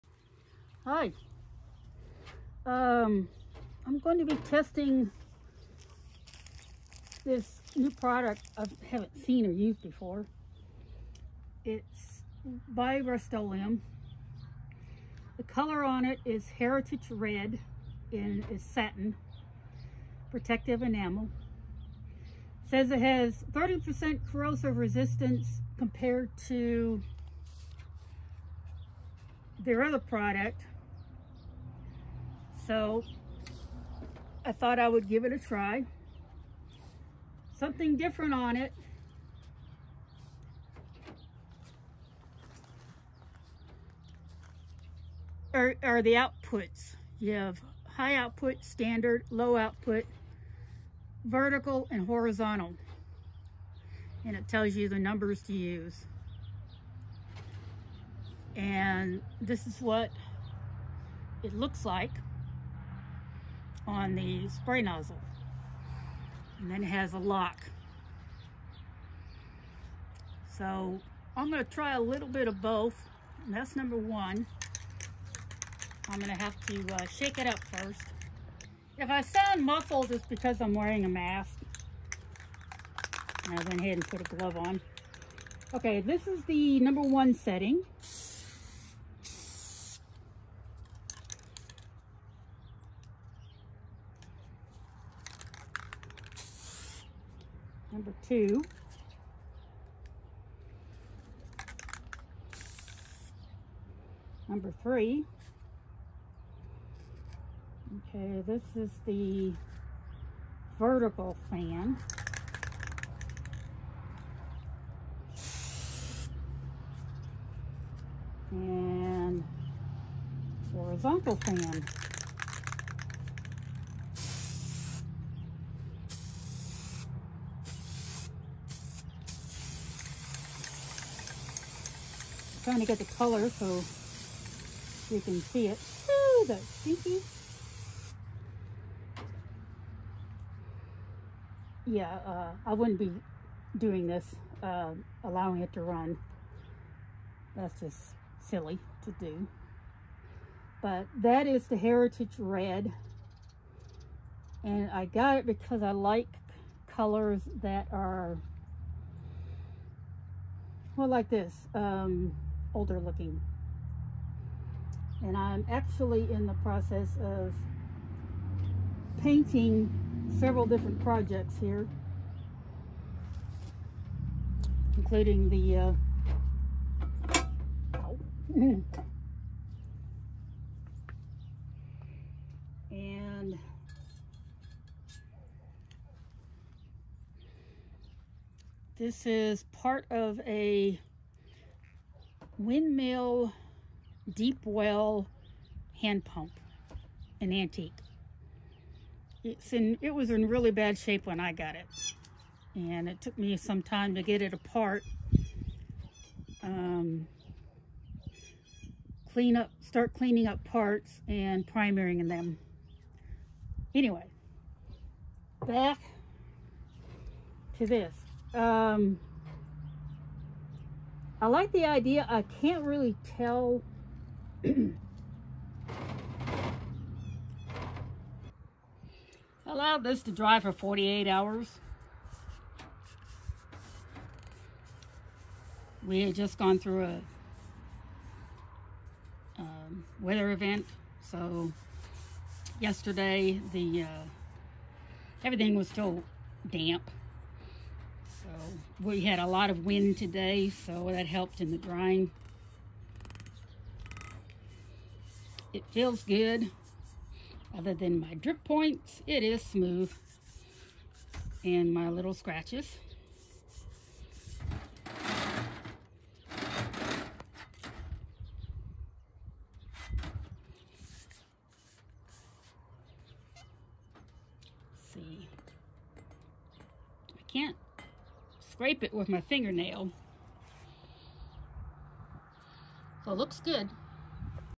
Rust-Oleum 5-1 Nozzle paint spray
Howdy y’all!
Thank you for dropping by, hope you find this helpful in your decision making.
Have another product I found at The Home Depot and made A video to share. Kinda obvious, right.
This is a new product from Rust-Oleum Brand Spray enamel paints. It has 30% more rust protection compared to the 2X sprays and has a 5 in 1 nozzle.
The color I chose is reminiscent of old barns and the like from decades past.
Because I had limited time I chose to paint the bottom of a tamper instead of my metal refurbishing projects.
Always wear protective clothing, gloves, mask and glasses. Overspray is a real **Bleep**! 🫢
The nozzle idea is pretty cool, in particular level 1(low output). Gives a better finish in the end. Level 2 is for the standard output and 3 is the high output when you are in a hurry .
There is also a vertical and horizontal option that operate on the regular output level(2).
To avoid accidental spraying by busy children, the spouse or THAT helpful friend, there is a lock position on the nozzle dial.
After running through my test sprays allowed the plate to dry 48 hours.
I tried scraping the edge but had no luck peeling or pricking the paint.
I like the color and I had not seen it beforehand, offered by Rust-Oleum.
I can give this a good recommendation for use on your projects, seems to Dry to a protective finish and doesn’tseem to require a top coat.